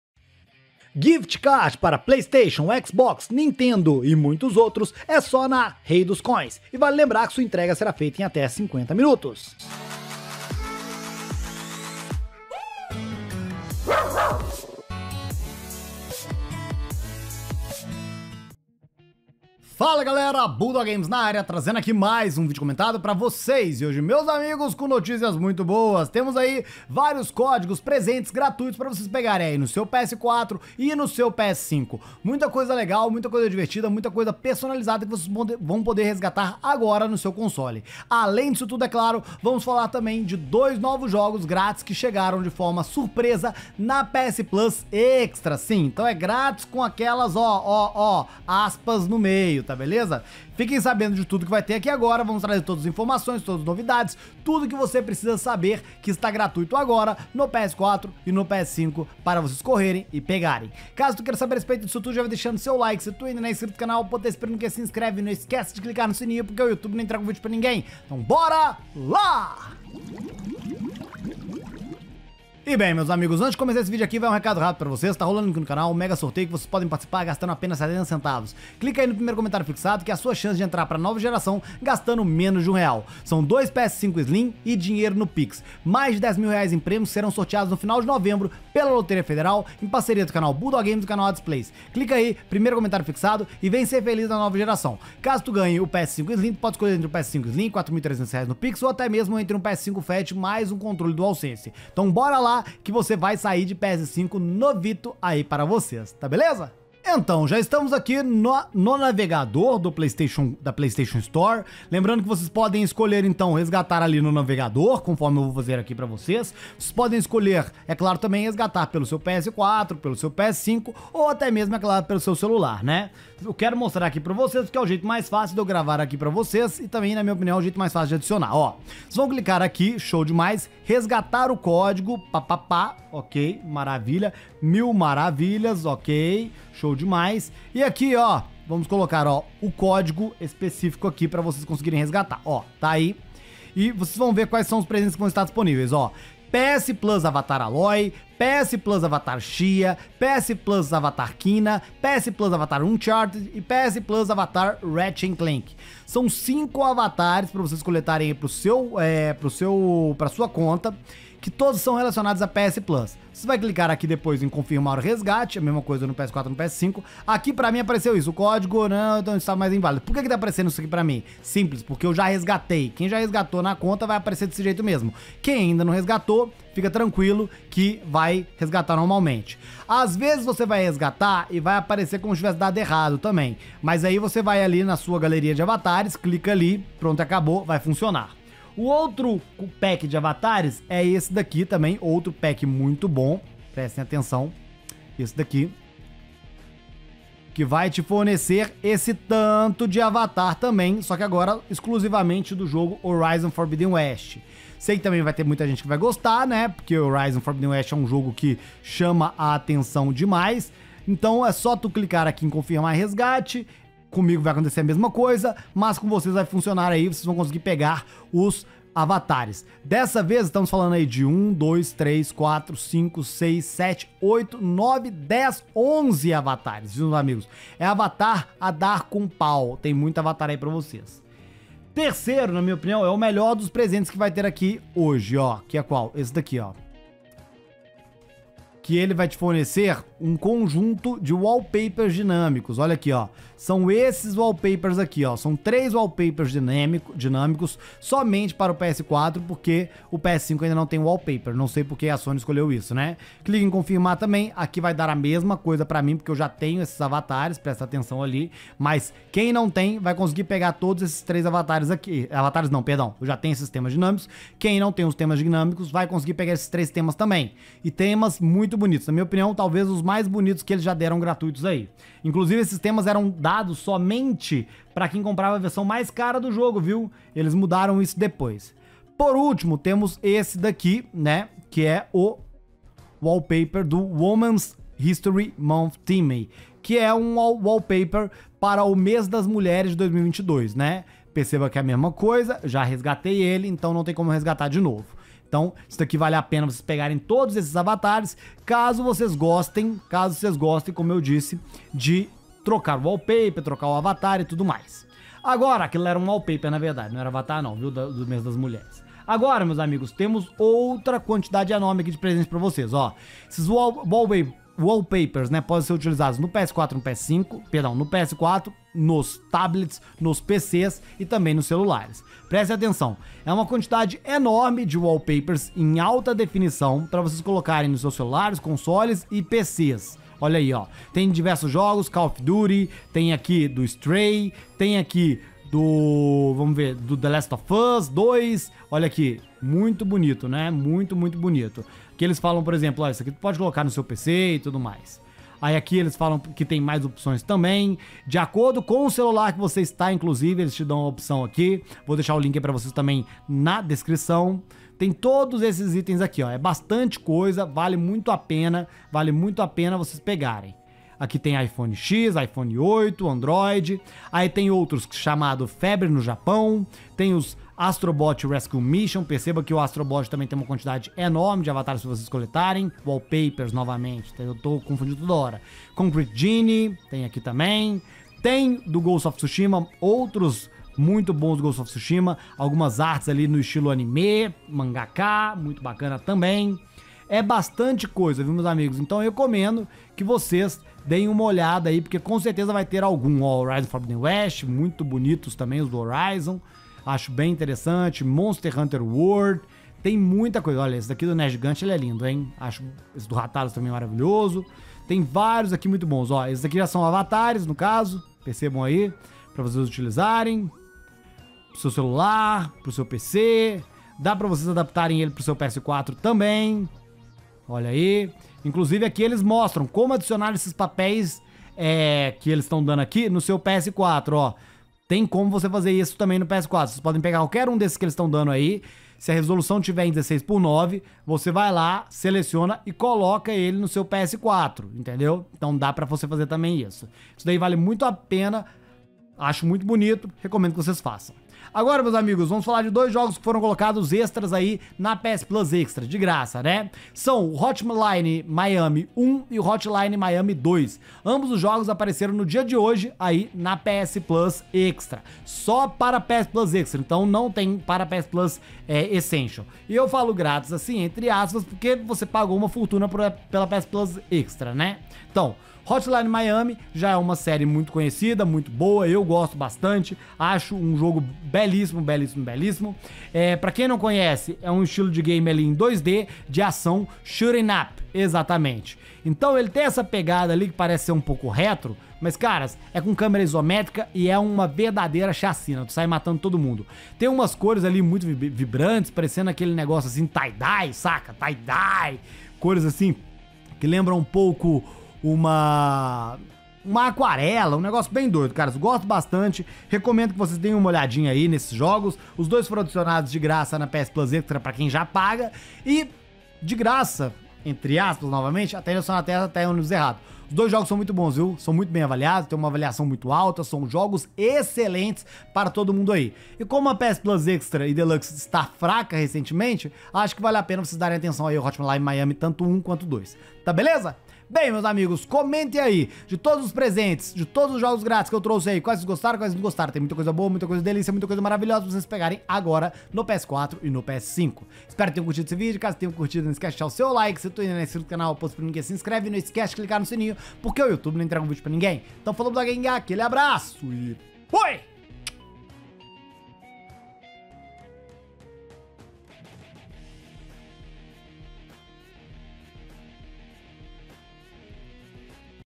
CORRE! CÓDIGOS GRÁTIS AGORA NO PS4 PS5! E 2 NOVOS JOGOS "GRÁTIS" NA PS PLUS EXTRA
América G7XK-TNNR-NPLL
Europa, Oriente Médio e África (EMEA): B2B9-X5NK-494R
América 5FM9-5LN6-FCM2
Europa 4PNM-4RNP-9C92
América 5DGE-AKNX-CGCD
Europa 6NN5-B2NK-XQM9